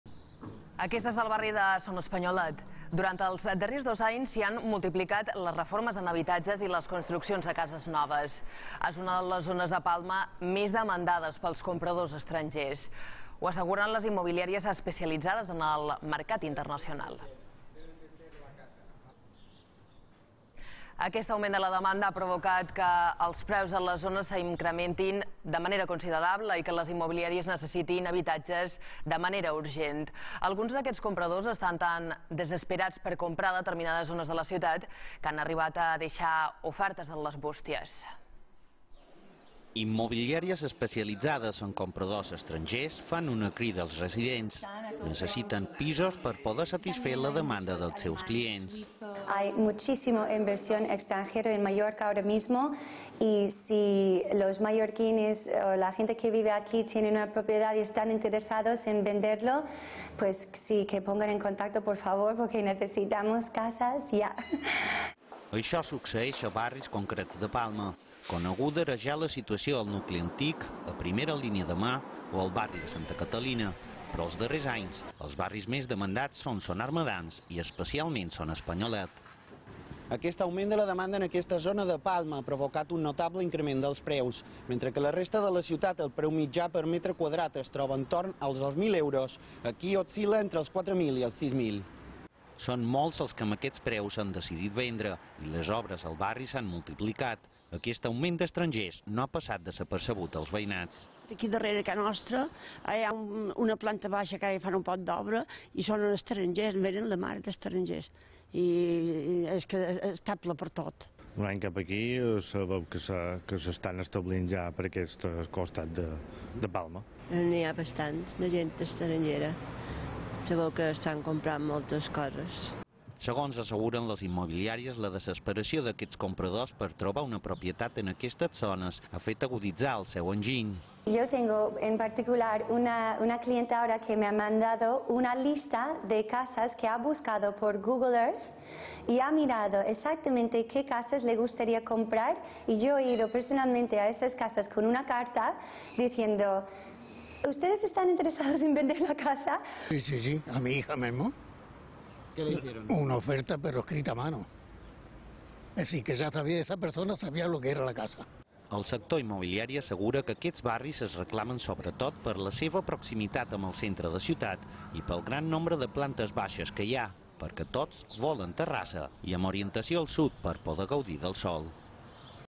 Immobiliàries especialitzades en mercat estranger necessiten habitatges en venda a Mallorca
L'augment de la demanda en determinades zones ha provocat que els preus s'incrementin considerablement i que les immobiliàries necessitin habitatges de manera urgent. Alguns d'aquests compradors estan tan desesperats per comprar en determinats barris de la ciutat, que han arribat a deixar ofertes en bústies.